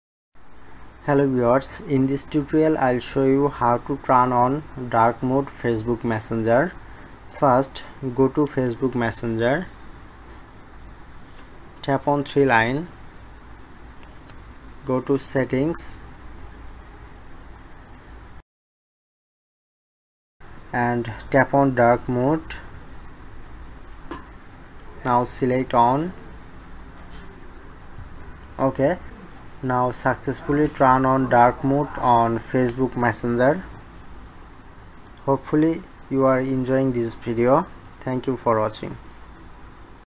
How to Turn on Dark Mode in Facebook Messenger 2024 | Enable FB Dark mode
Facebook Messenger's dark mode changes the app's typically bright white background to black, which is easier on the eyes and better for your battery.